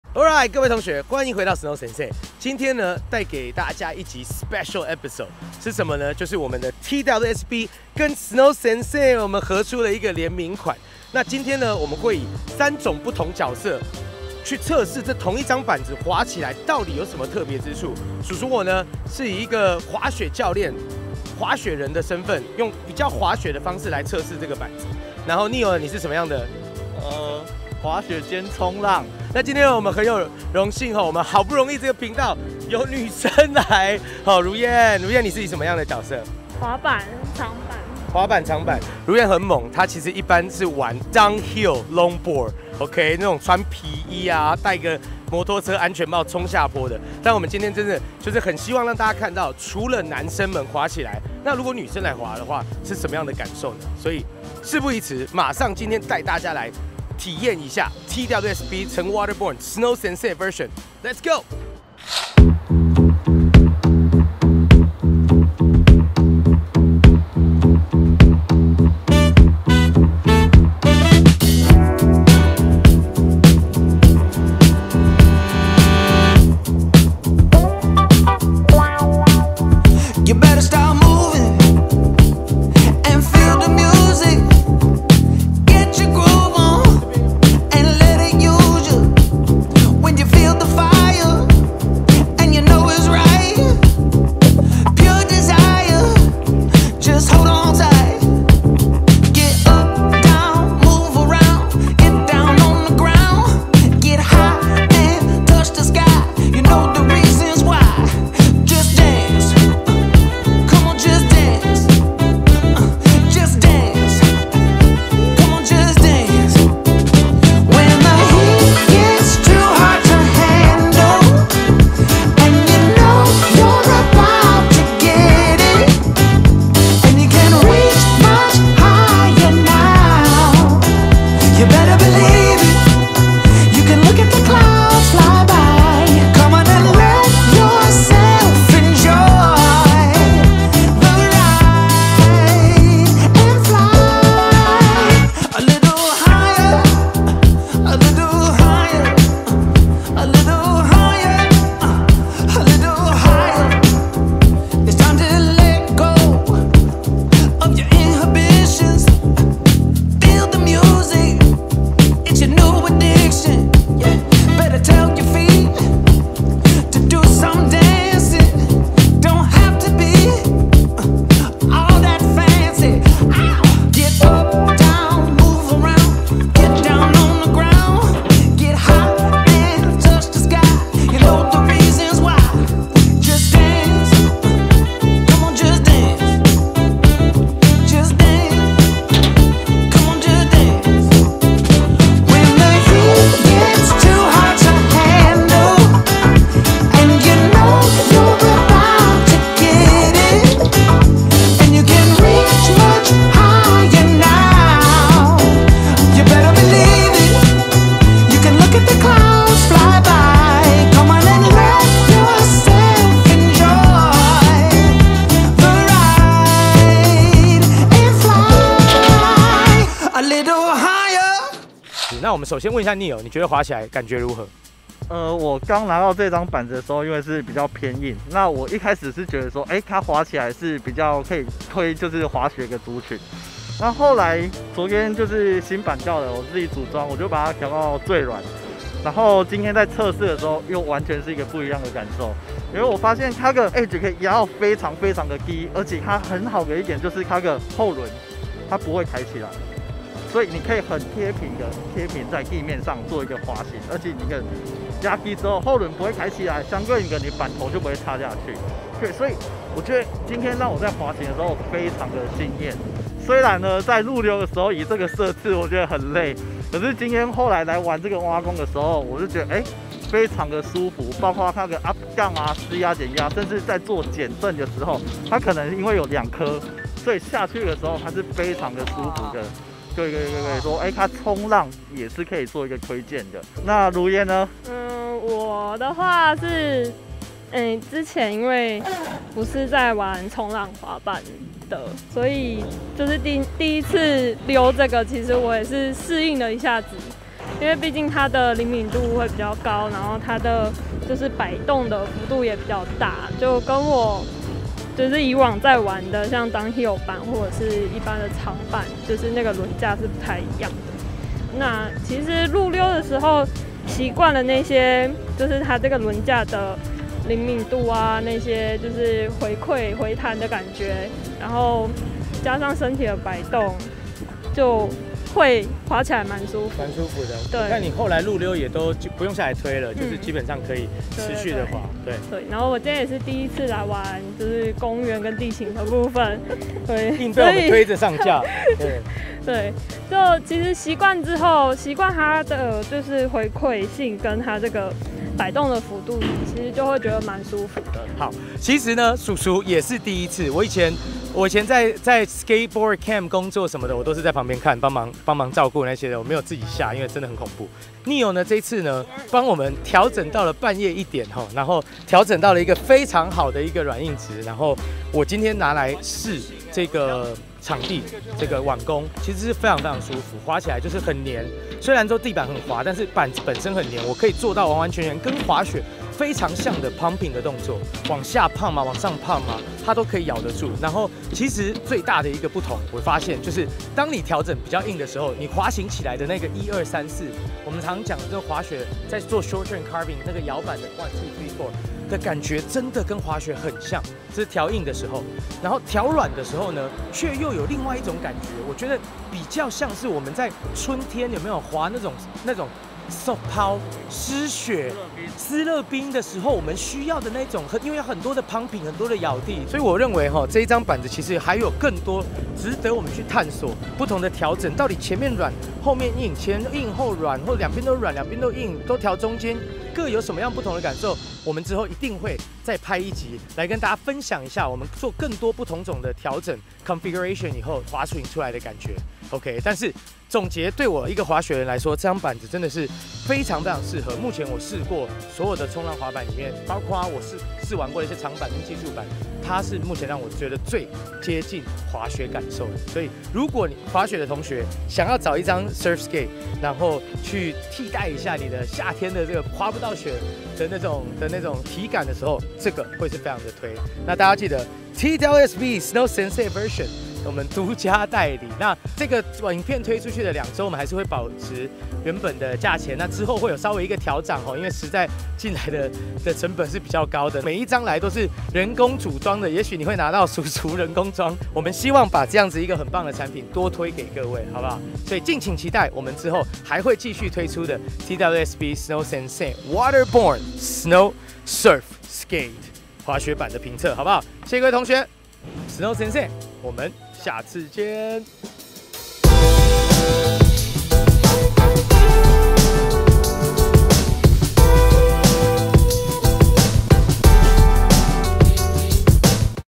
照照照過來！TWSB X SnowSensei 衝浪滑板專屬評測 | 非雪季訓練 Off-Season Training
是否記得曾在上一部衝浪滑板影片中亮過相的
由香港滑雪品牌TWSB所設計製作的衝浪滑板？
叔叔不僅僅是喜歡這張滑板，更進一步與TWSB合作
推出了——登楞！橘黑配色的SnowSensei特別板面
個性化板面，搭配最對滑雪人胃口的配置
讓你找回失落的腳感，更要讓你的非雪季訓練更上一層樓

想要更深入地認識這張衝浪滑板嗎？
除了Perry叔叔
今天還將邀請擅長衝浪與滑板的Neil
以及專攻下坡滑板與長板的如嫣
由兩位特別嘉賓，從不同的觀點出發，獻上他們對TWSB衝浪滑板的評測

心動就要馬上行動！
歡迎光臨SnowSensei選物商城

▲微信店&微信討論群組: thesnowsensei